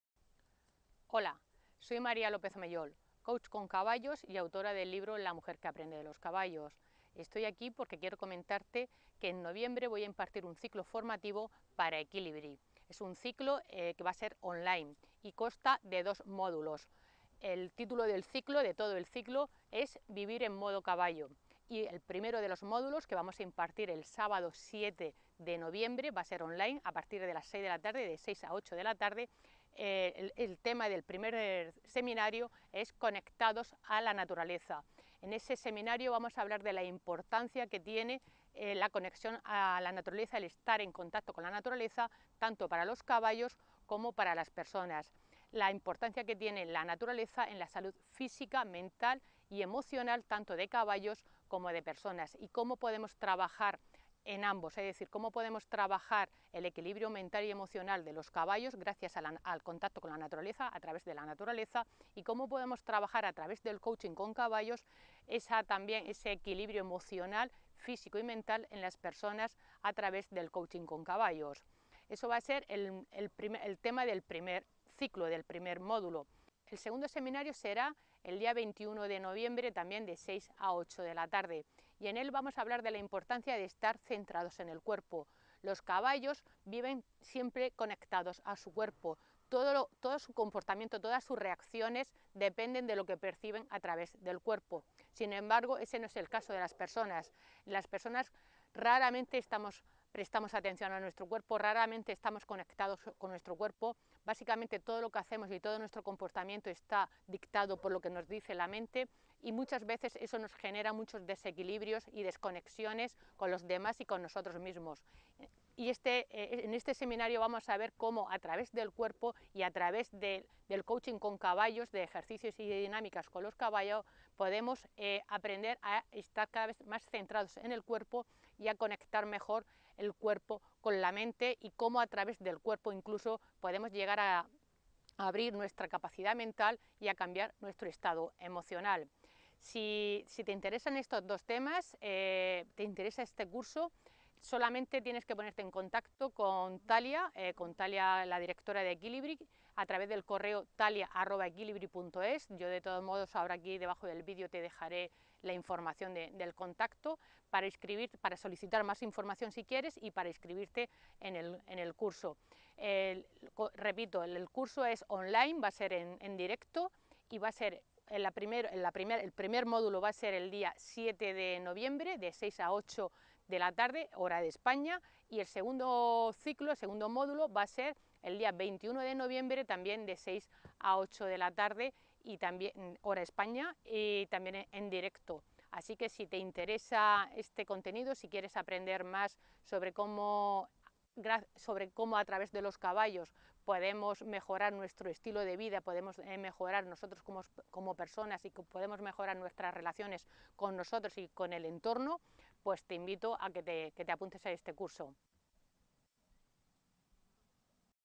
Presentación ciclo online vivir en modo caballo sobre coaching con caballos
𝗖𝗜𝗖𝗟𝗢 𝗙𝗢𝗥𝗠𝗔𝗧𝗜𝗩𝗢 𝗢𝗡𝗟𝗜𝗡𝗘 "𝗩𝗜𝗩𝗜𝗥 𝗘𝗡 𝗠𝗢𝗗𝗢 𝗖𝗔𝗕𝗔𝗟𝗟𝗢" 𝗘𝗡 𝗡𝗢𝗩𝗜𝗘𝗠𝗕𝗥𝗘

📌𝗦𝗮́𝗯𝗮𝗱𝗼 𝟳 𝗻𝗼𝘃𝗶𝗲𝗺𝗯𝗿𝗲 𝗱𝗲 𝟭𝟴 𝗮 𝟮𝟬 (𝗵𝗼𝗿𝗮𝗿𝗶𝗼 𝗘𝘀𝗽𝗮𝗻̃𝗮): 𝘊𝘰𝘯𝘦𝘤𝘵𝘢𝘥@𝘴 𝘢 𝘭𝘢 𝘯𝘢𝘵𝘶𝘳𝘢𝘭𝘦𝘻𝘢

Los caballos están conectados a la naturaleza y necesitan estar en contacto continuo con ella para estar sanos y equilibrados.

Cuando no están en la naturaleza o el contacto es escaso todas sus capacidades se limitan. ¿Y a las personas cómo nos afecta vivir alejadas de la naturaleza?
En este taller conocerás cómo influye el contacto con entornos naturales en la salud, carácter, comportamiento y aprendizaje de los caballos y… también de las personas.

📌𝗦𝗮́𝗯𝗮𝗱𝗼 𝟮𝟭 𝗱𝗲 𝗻𝗼𝘃𝗶𝗲𝗺𝗯𝗿𝗲 𝗱𝗲 𝟭𝟴 𝗮 𝟮𝟬 (𝗵𝗼𝗿𝗮𝗿𝗶𝗼 𝗘𝘀𝗽𝗮𝗻̃𝗮): 𝘊𝘦𝘯𝘵𝘳𝘢𝘥𝘰𝘴 𝘦𝘯 𝘦𝘭 𝘤𝘶𝘦𝘳𝘱𝘰

Los caballos viven centrados en su cuerpo. Todas sus reacciones y su comportamiento dependen de lo que perciben y registran a través de su cuerpo. Por eso no juzgan. Sienten y actúan en coherencia con ello.
Sin embargo, las personas apenas si le prestamos atención a nuestro cuerpo, a lo que siente y lo que nos dice.

Escuchamos sólo o básicamente los mensajes de la mente, que muchas veces son confusos y contradictorios precisamente porque la hemos desconectado de las sensaciones corporales.

En este taller veremos qué efectos tiene en nuestra vida vivir desconectados del cuerpo y qué pasaría si empezáramos a centrarnos en el cuerpo. Veremos cómo conectar cuerpo y mente y cómo los caballos nos pueden ayudar a ello.